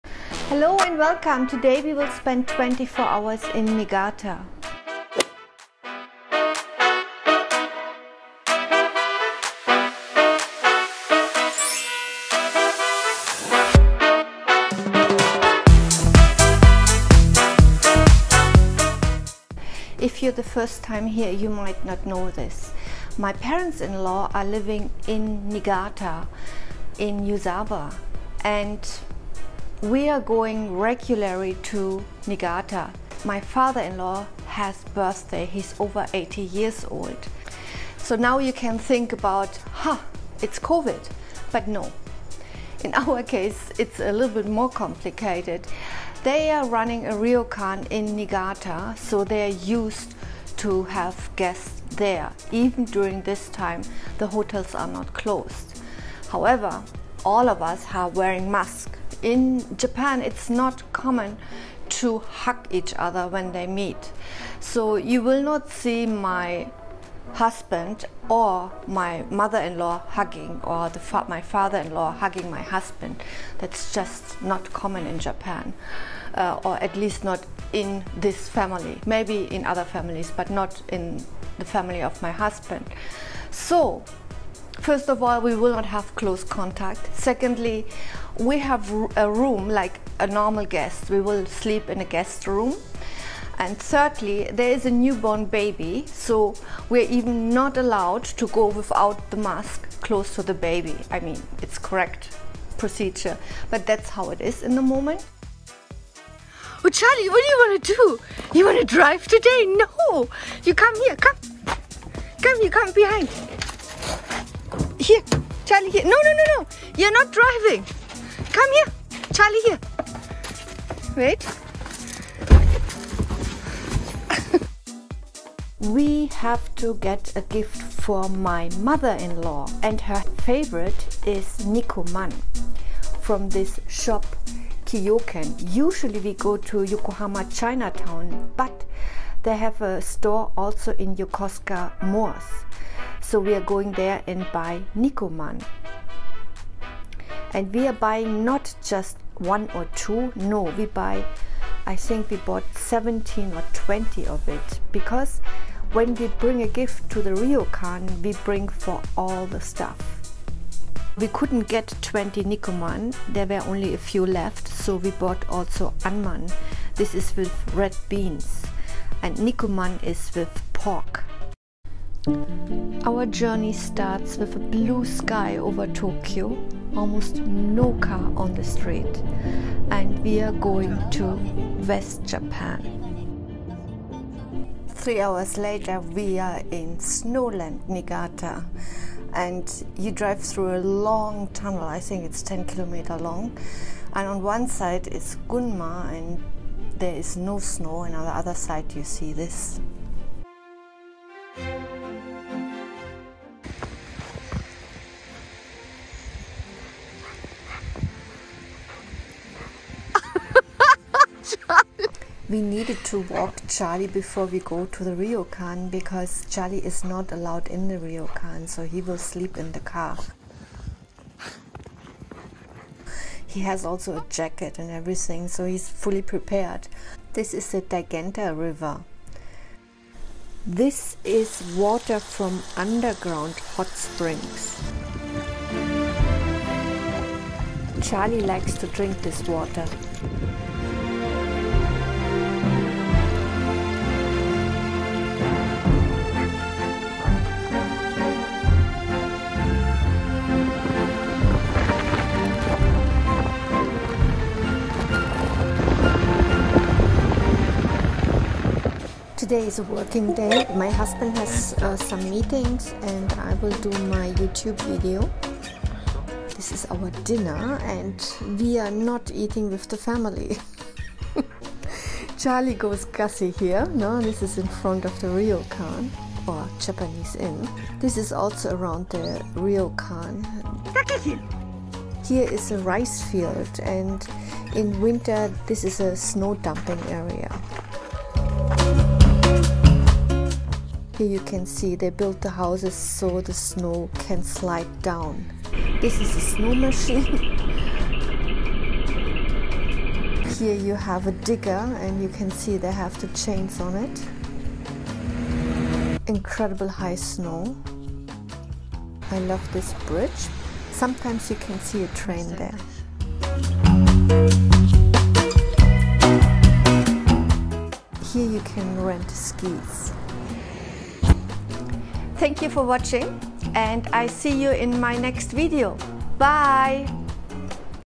A Day in Niigata
It is February and still cool white snow is piling up along the streets of Yuzawa in Niigata Prefecture. Echigo Yuzawa is also know as snow country if you want to see snow in Japan, this is a good choice. As most of my viewer know by now, we are a German Japanese couple and this is the town where my parents in law live. We are visiting for the birthday of my father in law. He turns 84 years old. We are spending a day in Niigata vlog and will enjoy traditional Japanese cuisine.

0:00 Beginning
0:05 Charly & Yuzawa's Uono river
0:22 Why I am visiting Niigata
1:46 Charly's place in the car
2:08 What kind of gifts we are bringing to Niigata / Japanese gifts to give
2:55 Journey starts
3:21 Daigenta river surroundings
3:51 Water from hot springs to melt snow
4:20 Our Ryokan or Japanese Inn Room
4:27 Ryokan Dinner
4:32 Walking Charly
4:40 Snow is piling up along the streets of Yuzawa
5:21 Ending

Music
Song: Artlist
Editing Software: imovie
Camera: Sony alpha 7C & iPhone 12Pro in 4k
Drone: DJI Mavic 2 Pro

XOX
Martina Takano
German living in Japan
vlog / blogger